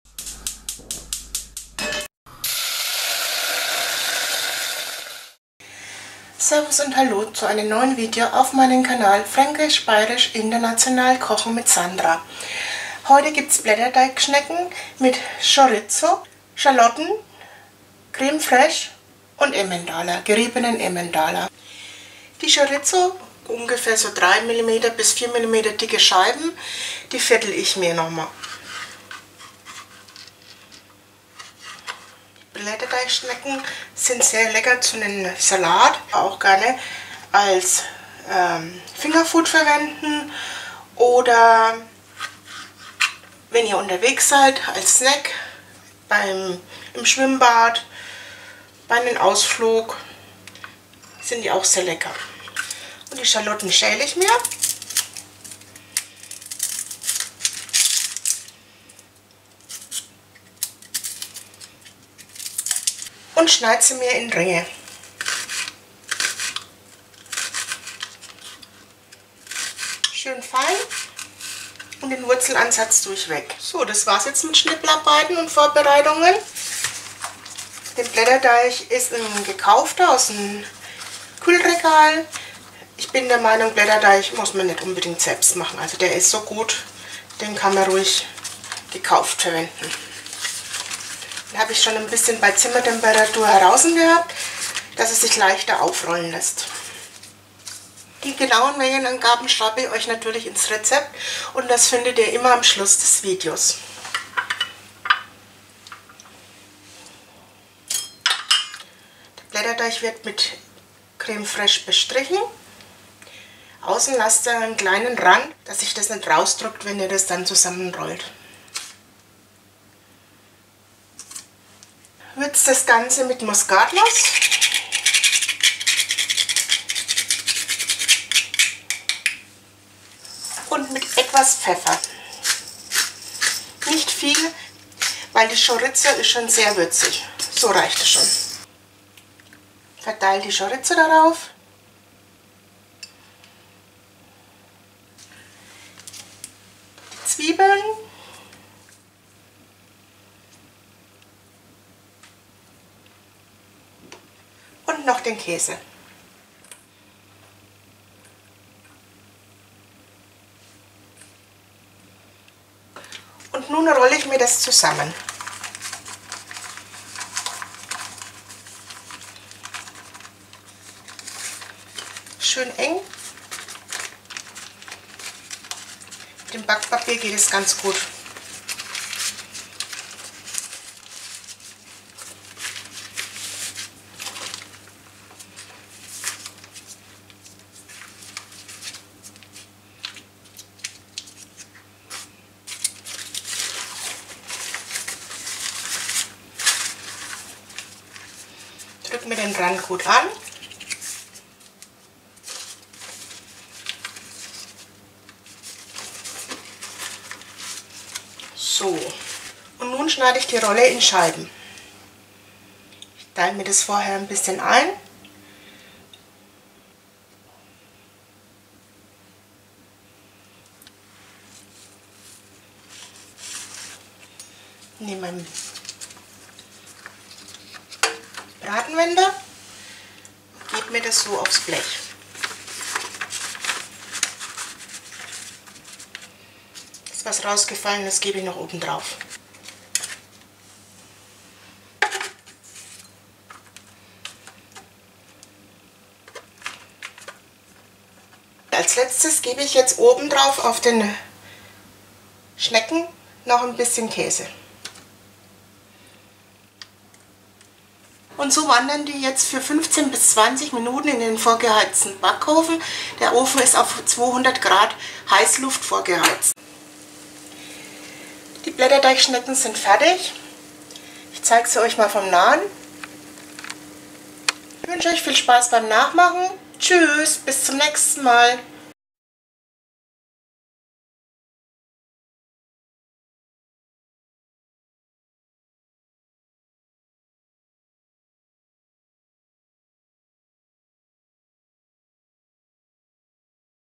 Chorizo - Schnecken, einfach, schnell, herzhaft, lecker
Chorizo - Schnecken sind sowohl einfach als auch schnell zubereitet. Egal ob kalt oder warm, schmecken sie einfach herzhaft gut.